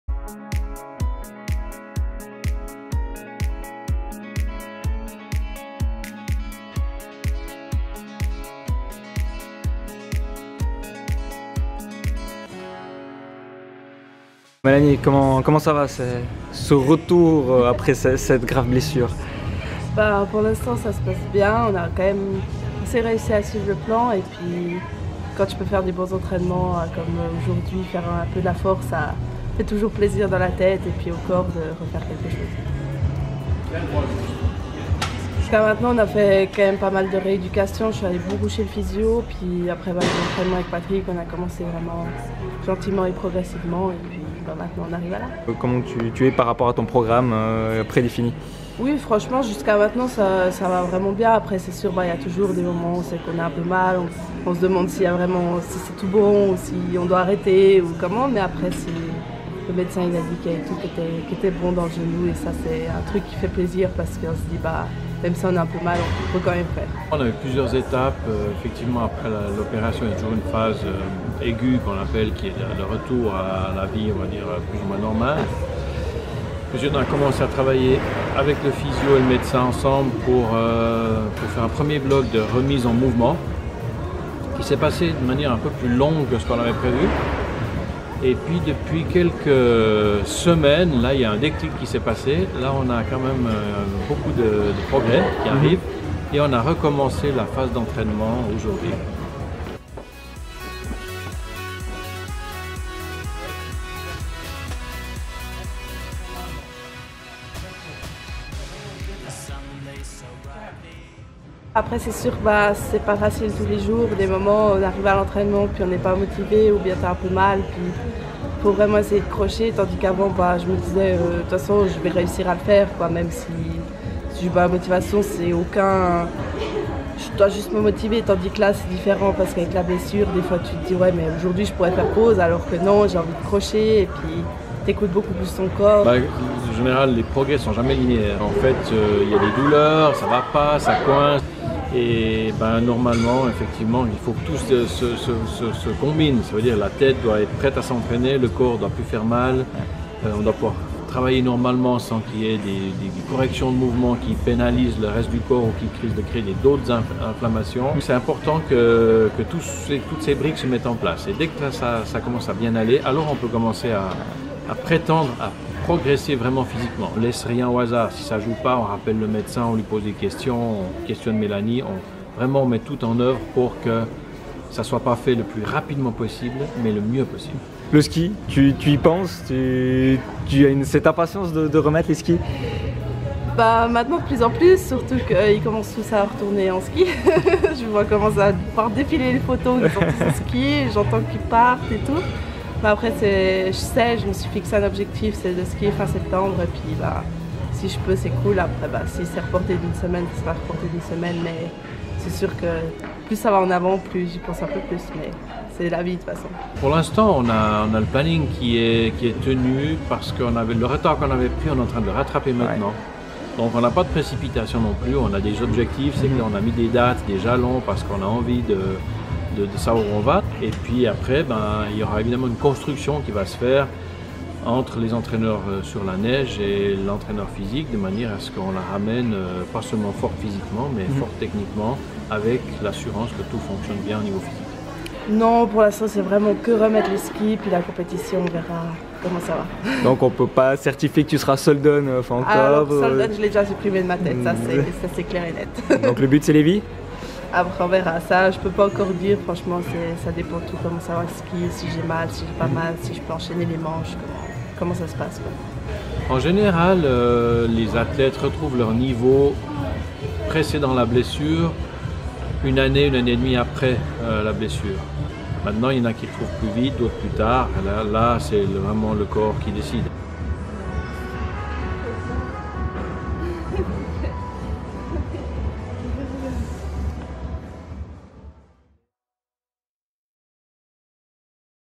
Mélanie Meillard: après la blessure, le plaisir de souffrir
Cinq mois après sa grave blessure au genou gauche, la skieuse Mélanie Meillard a retrouvé la salle de force pour préparer son nouvel hiver. La technicienne d'Hérémence et son préparteur physique Patrick Flaction se confient sur le long chemin de la rééducation.